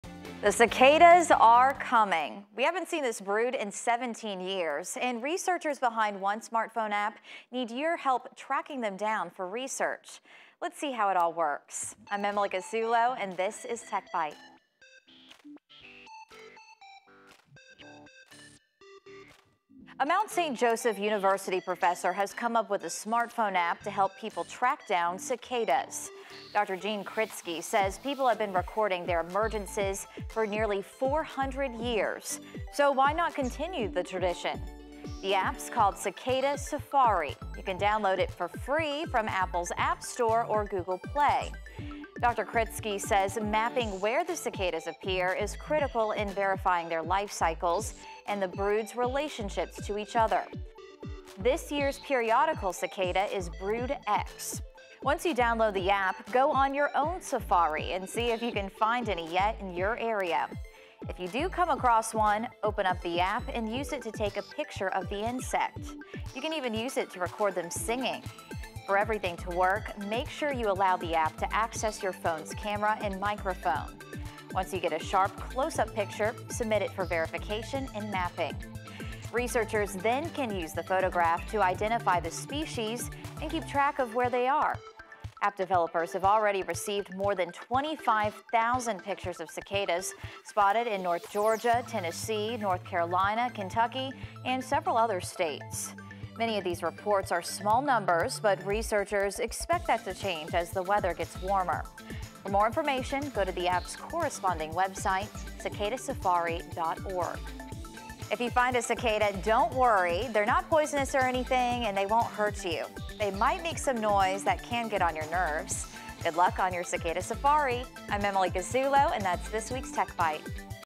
Tech for Tracking Cicadas
Researchers behind one smartphone app need your help tracking them down for research.